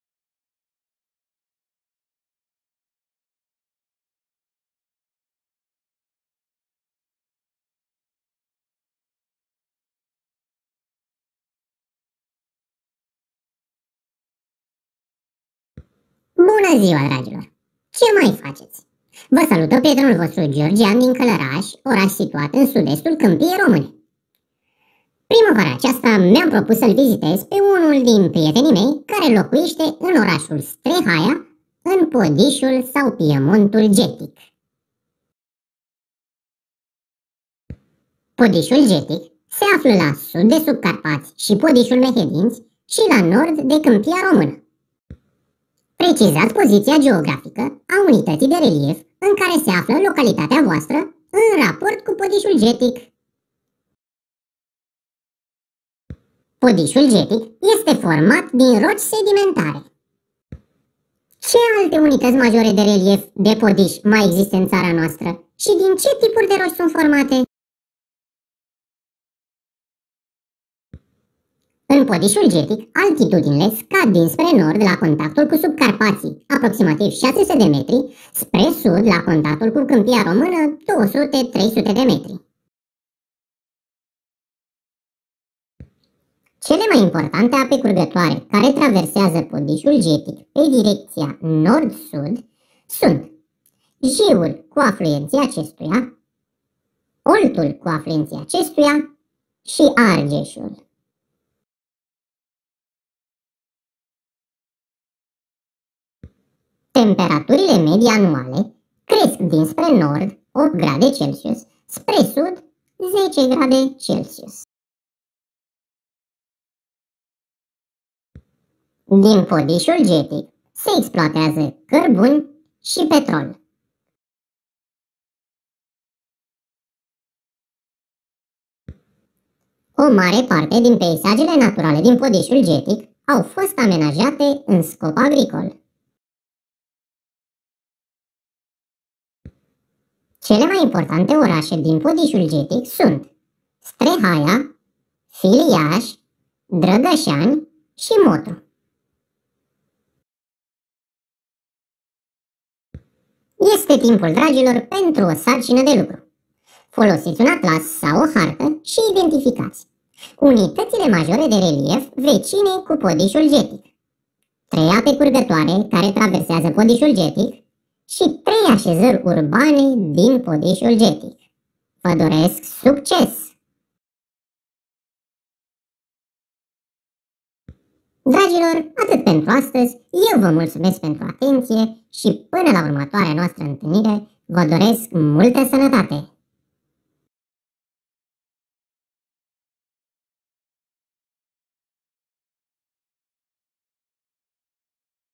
RED Podișul Getic. Geografie, clasa a IV-a
Bună ziua, dragilor!

    Această resursă educațională deschisă își propune dezvoltarea competenței 3.1. Identificarea poziției elementelor reprezentate pe hartă, utilizând metoda lucrului cu harta.
   Cu ajutorul acestei resurse educaționale deschise, elevii își formează competența specifică mai sus menționată într-un mod plăcut, fiindu-le expuse, sub forma unei povestiri animate, informații referitoare la Podișul Getic.
   La finalul acesteia le este propusă o temă de reflecție cu ajutorul căreia își pot fixa cunoștințele dobândite și pot continua demersul de formare al competenței specifice vizate.

Sursa hărților și a informațiilor - „Primul meu atlas geografic. Realitatea înconjurătoare explicată cu 40 de lecții video”, Mândruț O., Ungur G., Editura Corint, București, 2021
Peisaje agricole din Podișul Getic – Google Earth
Elemente grafice din biblioteca Microsoft Office 2019
Imagini și fotografii din arhiva personală.

     Resursa educațională propusă poate fi utilizată în timpul orelor de geografie din orice scenariu de învățare și, pe lângă dezvoltarea competenței specifice mai sus menționate, contribuie la dezvoltarea gândirii sistemice și analitice.
Se poate prezenta elevilor în diverse momente ale lecției sau poate fi propusă ca sarcină de lucru (temă pentru acasă), cu posibilitatea de a fi încărcată/încorporată pe orice tip de platformă educațională.
     În activitatea didactică, pentru a utiliza această resursă, profesorul are nevoie de un dispozitiv digital conectat la internet, de preferat, un televizor inteligent sau videoproiector, ecran de proiecție și boxe/difuzoare.

     Mulțumesc pentru atenție și vizionare!

     Sănătate și succes!